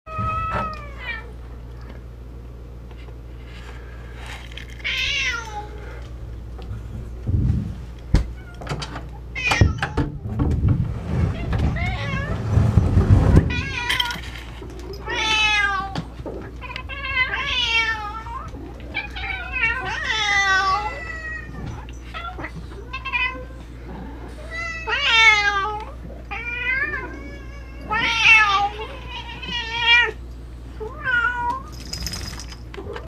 猫びっくり箱
引き戸を開けるとたくさんの猫が飛び出すよ！

（笑）

今日も猫は元気です。